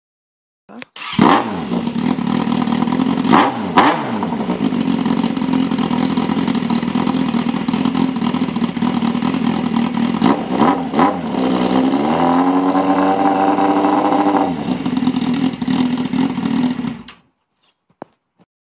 FZS 600 bez wydechu
sam kolektro bez wydechu, kreciłem tylko pod 3 tysiace bo bym mnie ludzie z sasiedztwa zatlukli, niedziela godzina 11